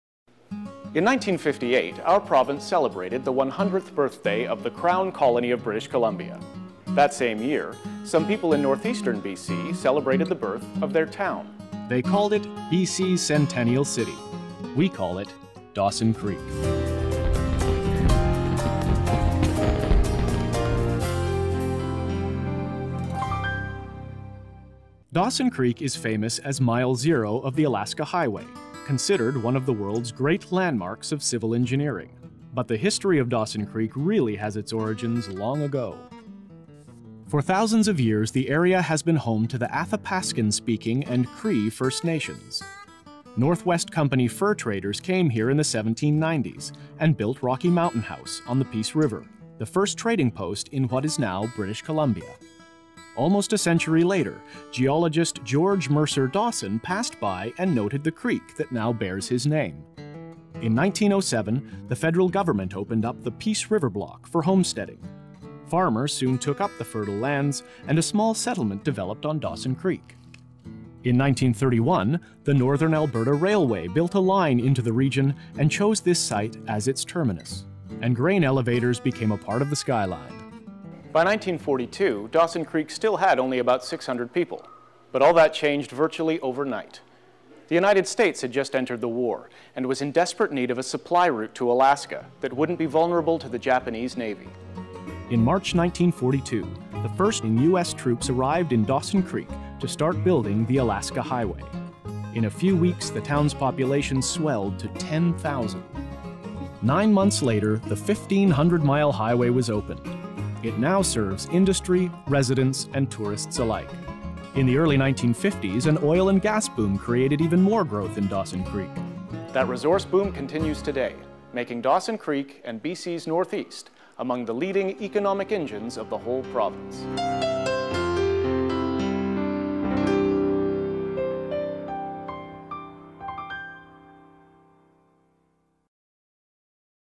Dawson Creek BC
In 2009 the BC Provincial Capital Commission did a series of short history docs about various aspects of BC.  With the end of the PCC in March of 2014 it is not clear what will happen to these videos so I thought would post them to ensure they remain online.

Mini-doc on Dawson Creek, best known as Mile 0 of the Alaska Highway and the regional centre of BC's Peace River district.
Here is the link the the BC PCC original video